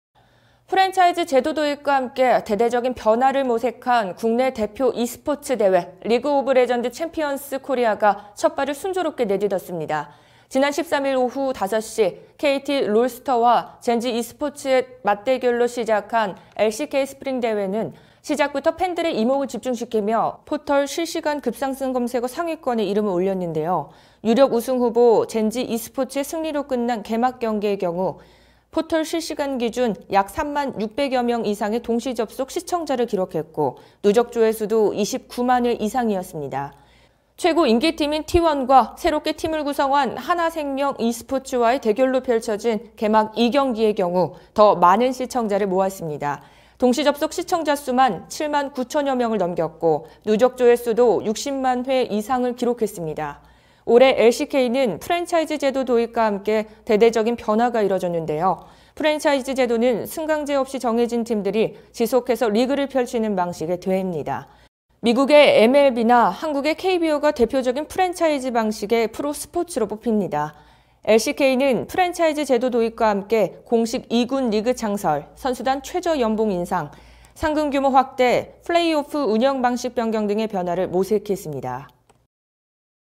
프랜차이즈 첫발 LCK, 개막전 ‘성공적’…3만6천 동접 기록 신동아방송뉴스
프랜차이즈 제도 도입과 함께 대대적인 변화를 모색한 국내 대표 e스포츠 대회 ‘리그오브레전드 챔피언스 코리아(LCK)’가 첫발을 순조롭게 내디뎠다.
지난 13일 오후 5시 kt 롤스터와 젠지 이스포츠의 맞대결로 시작한 LCK 스프링 대회는 시작부터 팬들의 이목을 집중시키며 포털 실시간 급상승 검색어 상위권에 이름을 올렸다.
유력 우승 후보 젠지 이스포츠의 승리로 끝난 개막경기의 경우 포털사이트 기준 약 3만600여명 이상의 동시접속 시청자를 기록했고 누적 조회수도 29만회 이상이었다.
최고 인기 팀인 T1과 새롭게 팀을 구성한 한화생명 e스포츠와의 대결로 펼쳐진 개막 2경기의 경우 더 많은 시청자를 모았다.
동시접속 시청자수만 7만9000여명을 넘겼고 누적 조회수도 60만회 이상이었다.
올해 LCK는 프랜차이즈 제도 도입과 함께 대대적인 변화가 이뤄졌다.
프랜차이즈 제도는 승강제 없이 정해진 팀들이 지속해서 리그를 펼치는 방식의 대회다.
미국의 MLB나 한국의 KBO가 대표적인 프랜차이즈 방식의 프로 스포츠다.
LCK는 프랜차이즈 제도 도입과 함께 공식 2군 리그(LCK CL) 창설, 선수단 최저 연봉 인상, 상금 규모 확대, 플레이오프 운영 방식 변경 등의 변화를 모색했다.